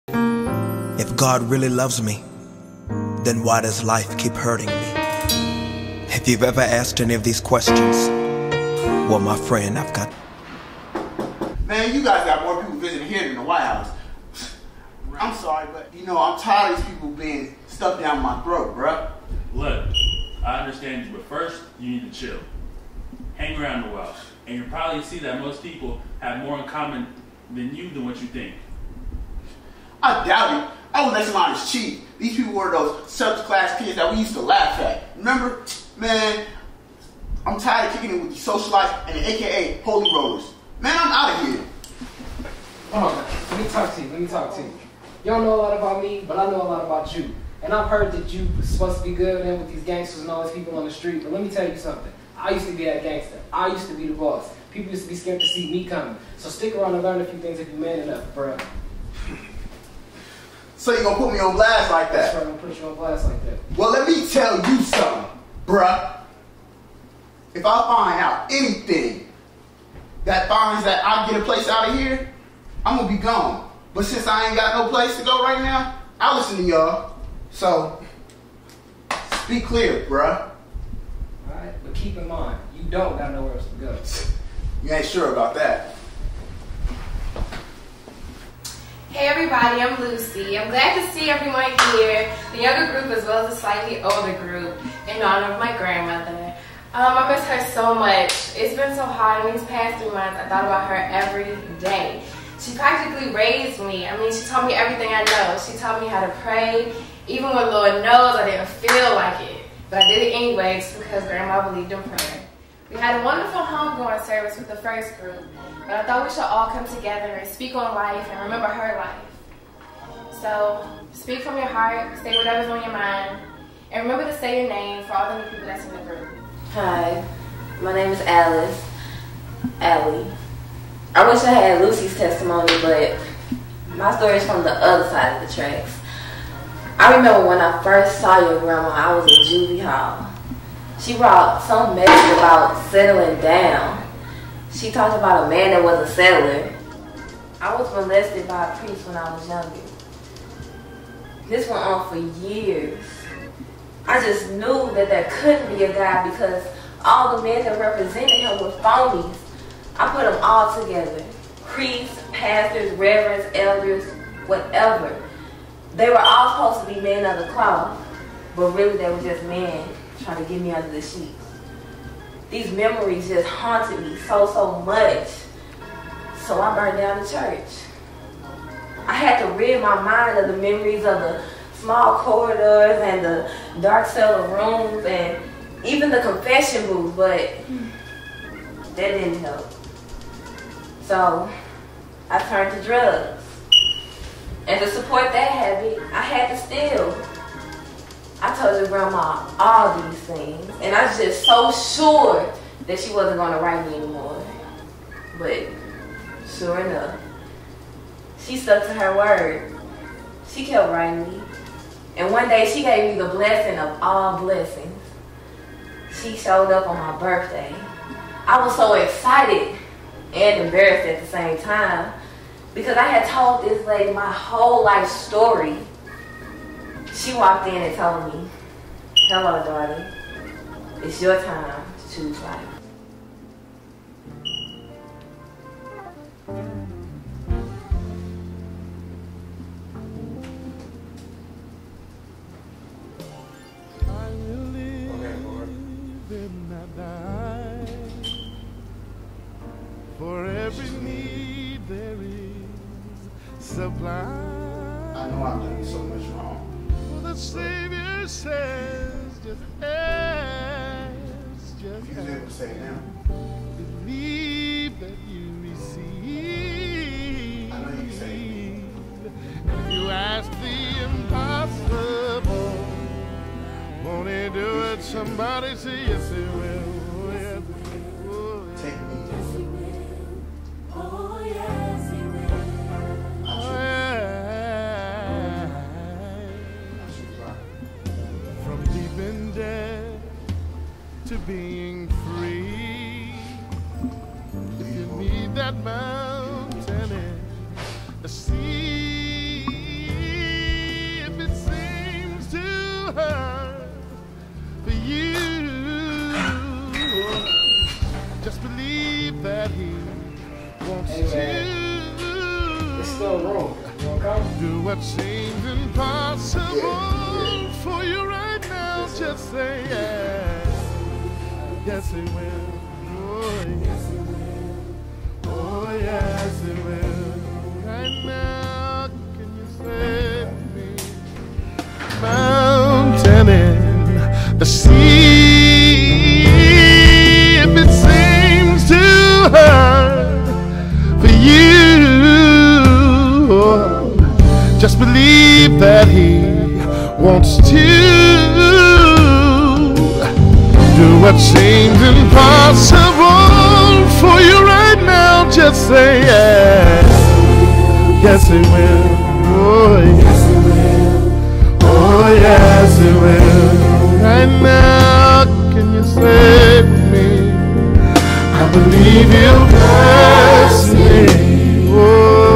Life or Death (AUC Schools 2009)
Created @ AUC Schools in 2009  as part of Campus MovieFest, the worlds largest student film festival.